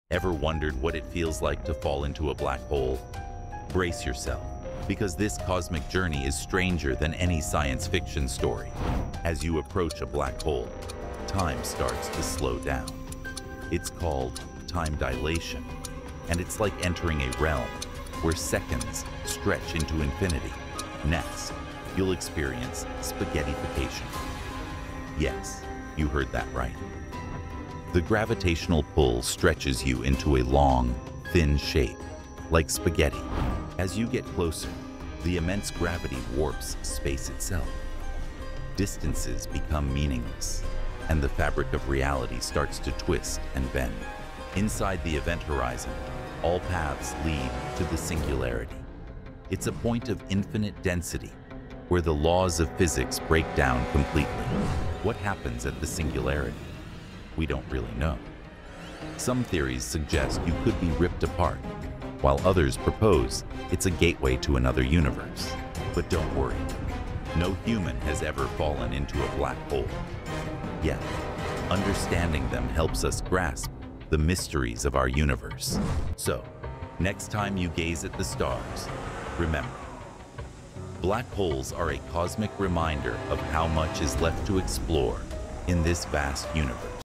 The Mind-Bending Journey into a Black Hole! 🌌✨
Ever wondered what it's like to fall into a black hole? Join us on this cosmic adventure where time slows down, and gravity stretches you like spaghetti! Discover the mysteries of time dilation, spaghettification, and the enigmatic singularity. What lies beyond the event horizon? Let's explore the wonders of our universe together!